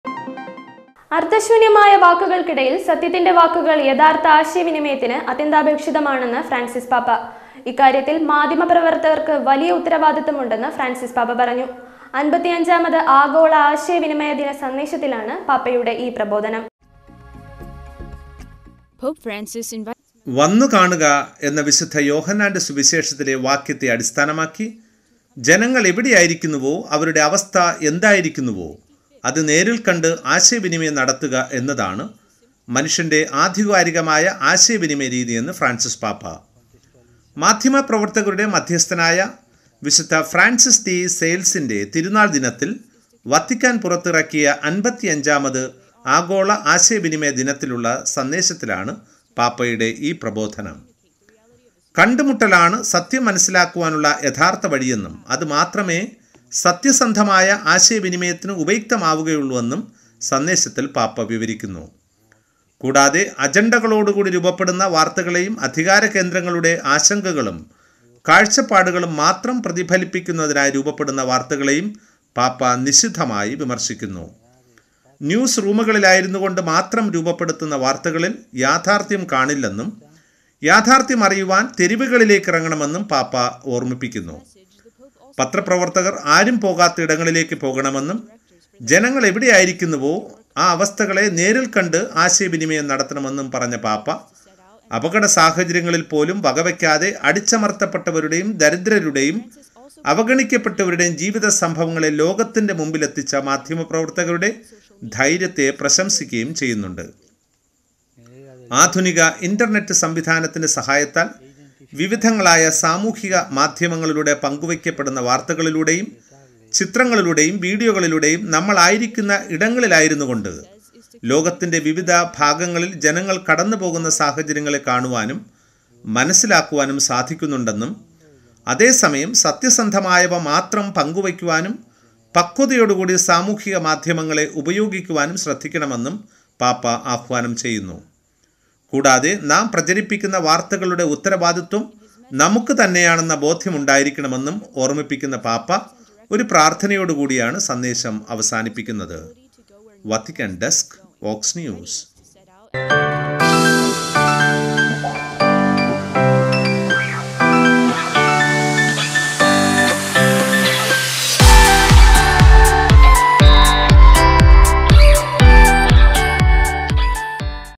ന്യൂസ് റൂമുകളില്‍ മാത്രം ഉടലെടുക്കുന്ന വാര്‍ത്തകളില്‍ യാഥാര്‍ത്ഥ്യം കാണില്ല : പാപ്പ
വാർത്തകൾ നിങ്ങളുടെ വാട്ട്സ് ആപ്പിൽ ലഭ്യമാക്കുവാൻ നിങ്ങൾ ആഗ്രഹിക്കുന്നുവൊ? എങ്കിൽ ഞങ്ങളുടെ ഈ ഗ്രൂപ്പിൽ ക്ലിക്ക് ചെയ്ത് പങ്ക് ചേരുക